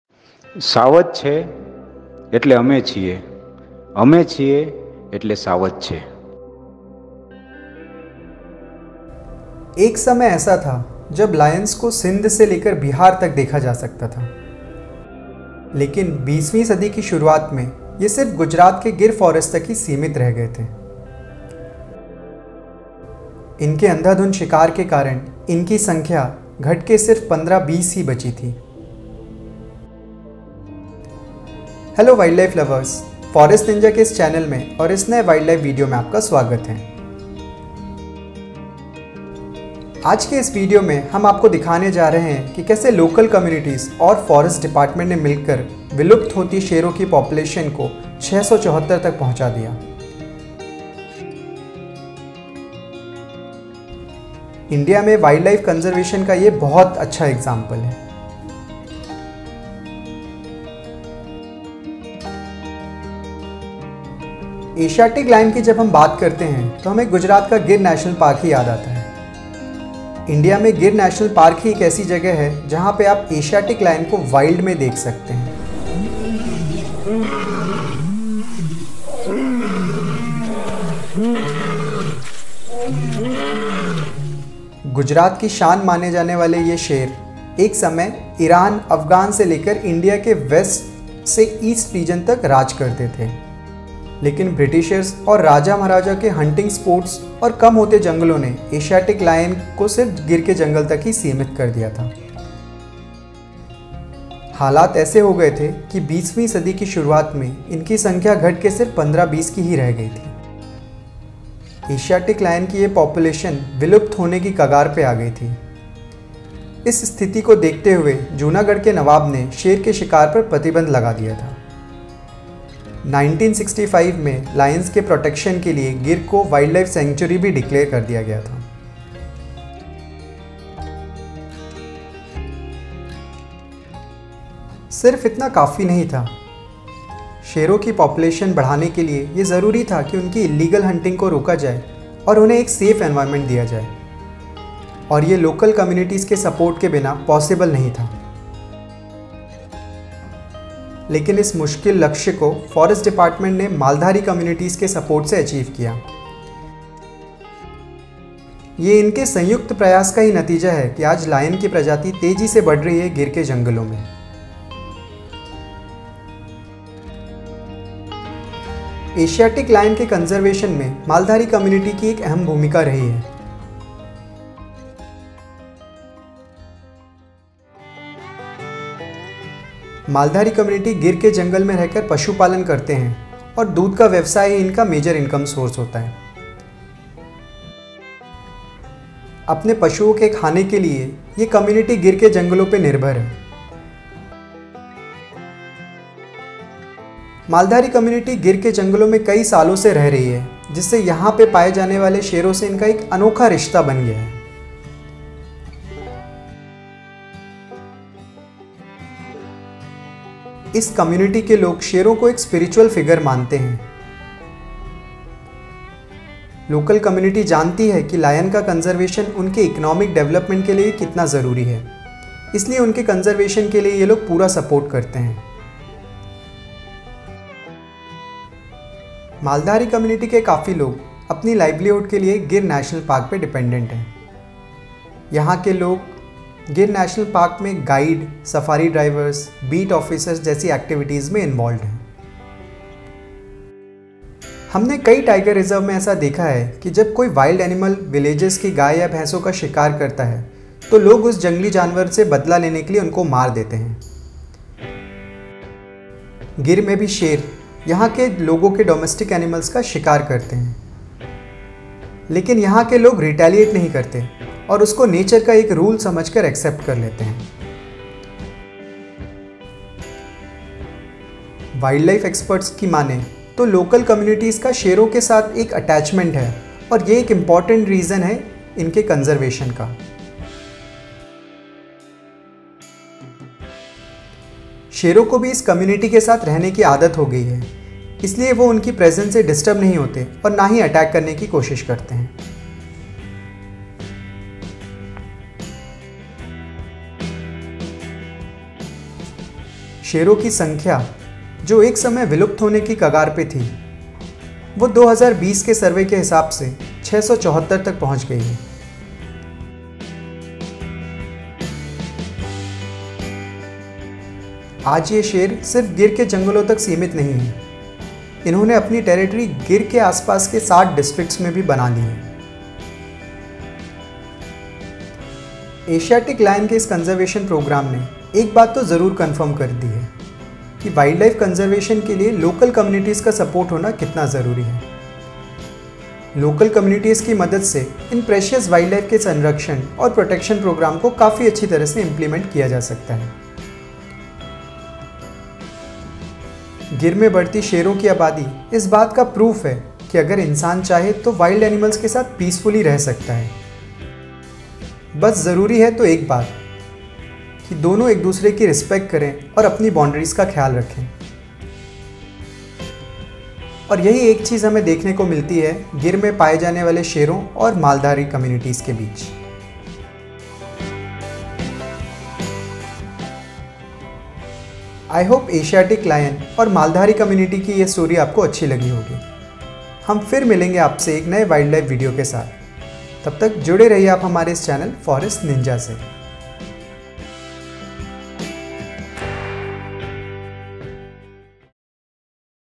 The Asiatic Lion Conservation Story | Gir National Park| Forest Stories| Wildlife
The Maldharis are a traditional pastoralist community found primarily in the state of Gujarat, India. There is a significant and unique relationship between the Asiatic lion and the Maldharis community due to their coexistence in the Gir Forest National Park and its surrounding areas.

The Maldharis have been living in close proximity to the lions for generations and have developed a symbiotic relationship with these big cats. The Maldharis have been sharing their habitat with the lions for centuries. They have adapted their lifestyle and practices to coexist with these apex predators. The Maldharis are primarily involved in livestock rearing, and they graze their cattle and buffaloes in the same areas where lions roam. This creates a unique scenario where humans and lions share the same space.

The Maldharis play a role in the conservation of Asiatic lions by acting as informal guardians of the lions and their habitat. They often help in reporting lion sightings and other important information to authorities. The lions have cultural and religious significance for the Maldharis. They consider the lions to be sacred and a symbol of pride.

The coexistence of the Maldharis and the Asiatic lions in the Gir Forest is considered a conservation success story. Efforts to protect and manage the lion population have led to an increase in their numbers over the years.